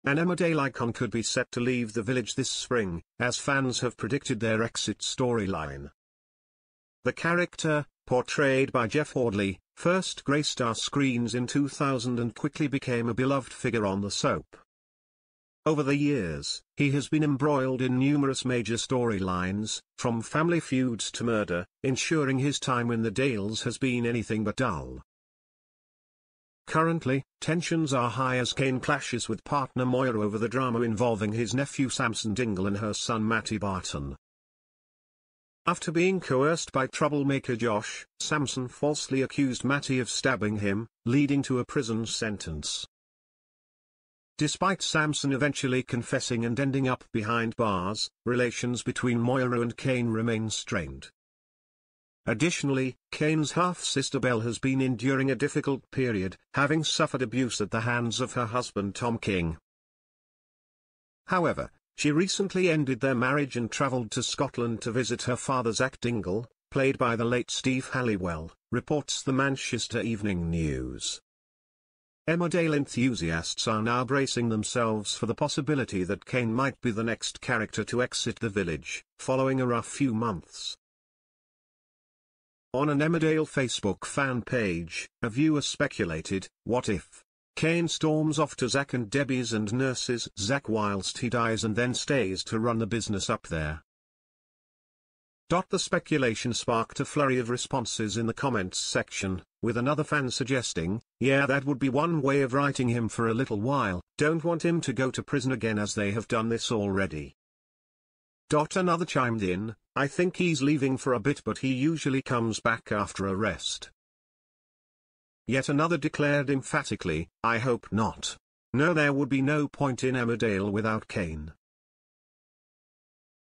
ITV Emmerdale Iegend's exit 'seaIed' 24 years after debut in heartbreaking twist
An Emmerdale Iegend could be bidding fareweII to the village.